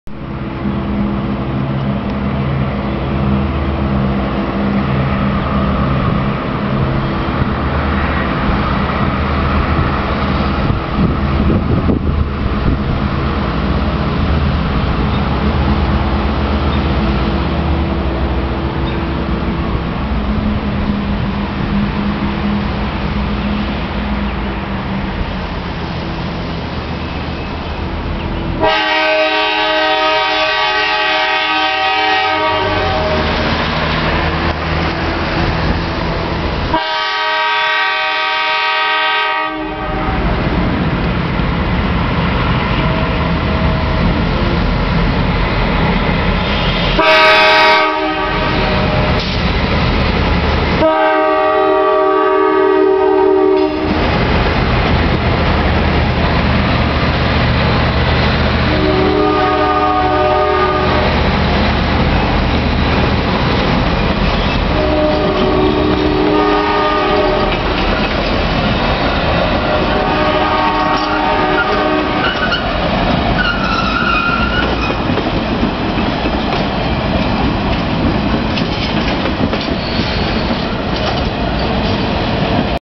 NS 955, the inbound KY Derby OCS at Harrodsburg Ky just before dawn 04/25/10
Video by Carmon Harrod